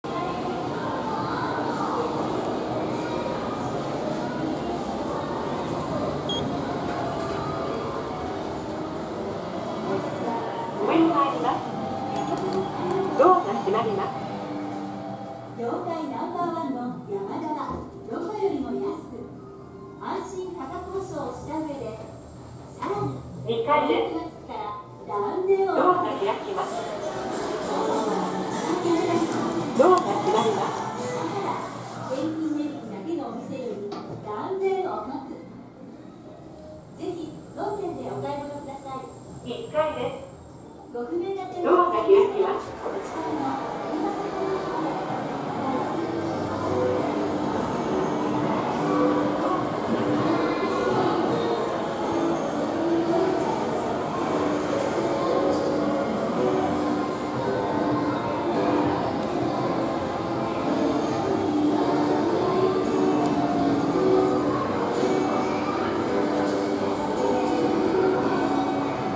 ヤマダ電機三原店のエレベーターFujitec製・15名・1000kg
ヤマダ電機三原店のエレベーターです。フジテック製・乗用15名・積載1000kg